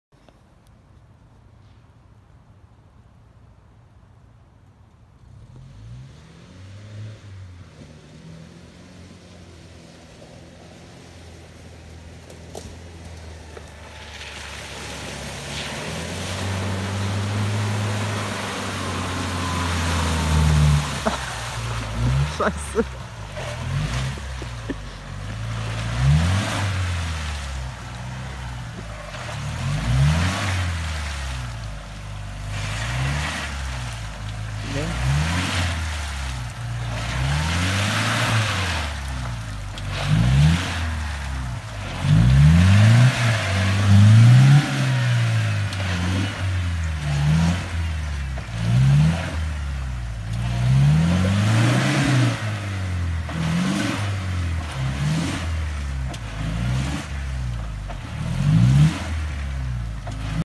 Opel Frontera fast festgefahren im Wald / Spaß am Sonntag / Rügen / Samtens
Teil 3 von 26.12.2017